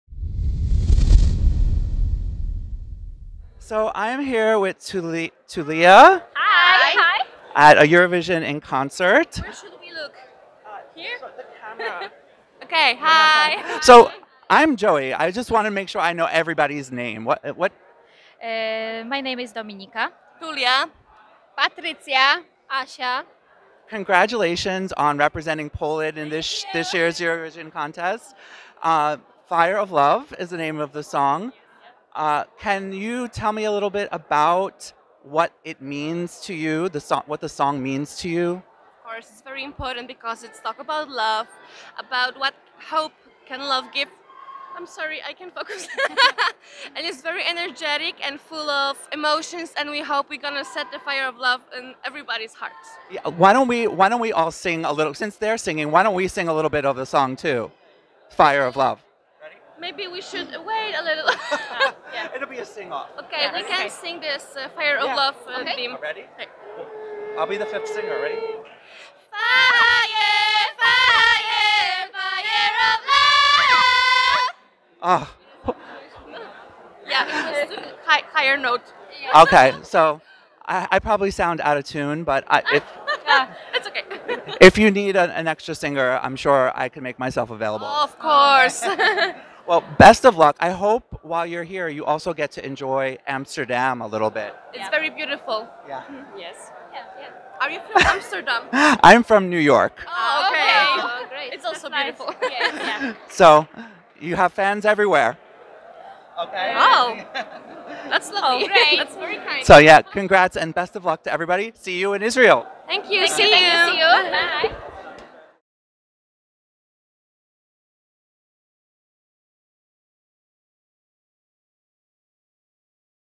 Eurovision 2019: Interview with Tulia at Eurovision in Concert | Poland
Poland's Tulia talks to ESCplus at Eurovision in Concert 2019 in Amsterdam.